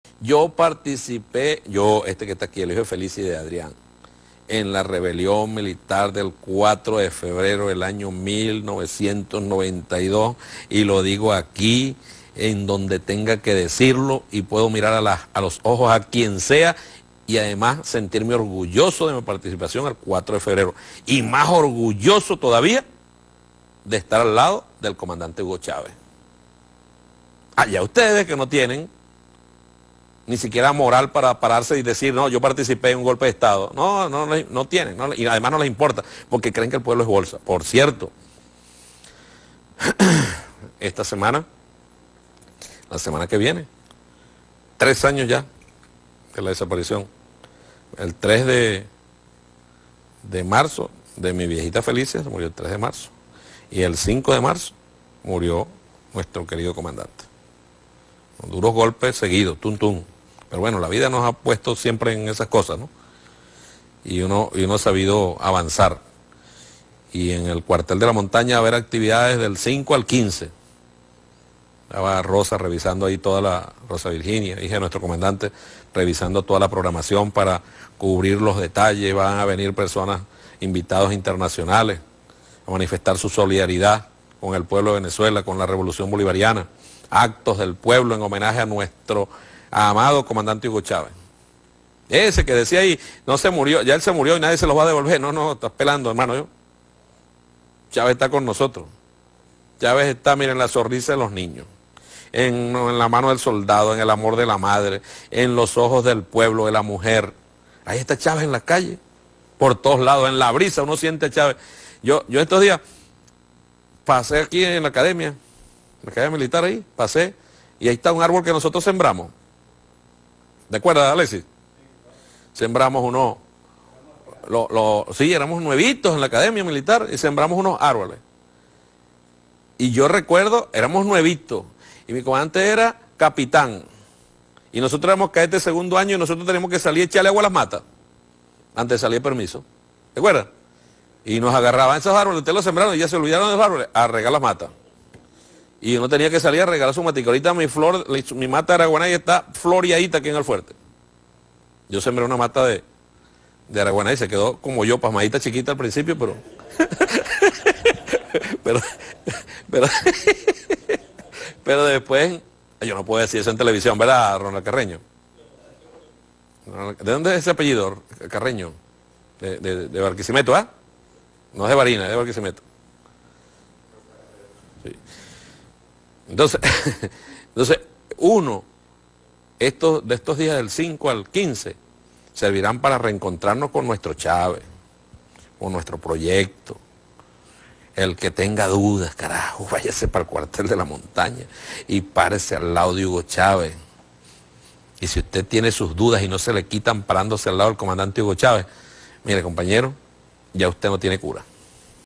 Cabello: En la brisa uno siente a Chávez
ND / 24 feb 2016.- El diputado y primer vicepresidente del Psuv, Diosdado Cabello, aseguró este miércoles que el fallecido presidente Hugo Chávez y su legado continúan más vivos que nunca y se pueden sentir "por todos lados".

Así lo dijo en los minutos finales de su programa de los miércoles mientras se refería a dos declaraciones, una de Napoleón Bravo en la que aseguraba que él nunca se interesó por la política, y otra de Henrique Capriles cuando, al dirigirse a los chavistas, les dijo: "El presidente Chávez ya no esta y nadie se los puede devolver".

"Mire", dijo Cabello. "Yo participé... en la rebelión militar del 4 de febrero del año 1992 y lo digo aquí...y además, me siento orgulloso de mi participación el 4 de febrero y más orgulloso todavía, de estar al lado del comandante Hugo Chávez... allá ustedes que no tienen ni siquiera moral para pararse y decir, sí, yo participé en un golpe de Estado".

Anunció que entre el 5 y el 15 de marzo se conmemorará el tercer aniversario del fallecimiento del presidente Hugo Chávez. "Ése que decía ahí - en referencia a Capriles - ya se murió y nadie se los va a devolver, no, no. Estás pelando hermano. Chávez está con nosotros. Chávez está en la sonrisa de los niños, en la mano de soldado, en el amor de la madre, en los ojos del pueblo, de la mujer. Ahí está Chávez, en la calle. Por todos lados. En la brisa uno siente a Chávez".